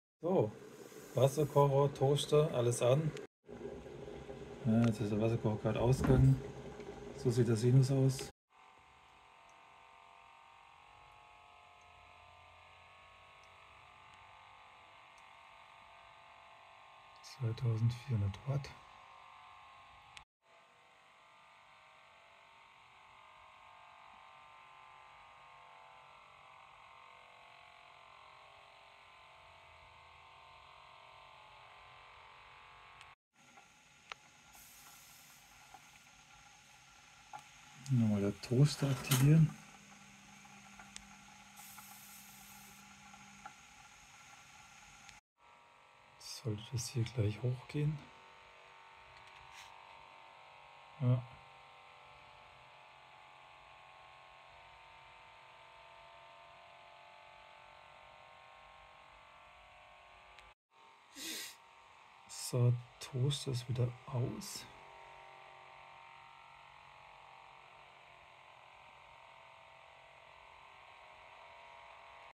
Neue SINUS Messung Voltronic 5000W Off Grid Hybrid 48V Inverter dem China Taiwanesen unrecht getan?
Neue SINUS Messung Voltronic 5000W MPPSolar (und andere) Off Grid Hybrid 48V Inverter dem China Taiwanesen unrecht getan?
die SINUS kurve wird etwas flacher aber im Gegensatz zur letzten Messung, wo versucht wurde ein eKleinstfahrzeug zu laden: keine Verzerrung des SINUS 😎👍🏻🌻👌🏻☀️☀️☀️ way to go
PS: Auch der Betrieb von 2000W Verbrauchern wie Spühlmaschine sind KEIN Problem :D LET THE SUN SHINE!😎👍🏻🌻👌🏻☀️☀️☀️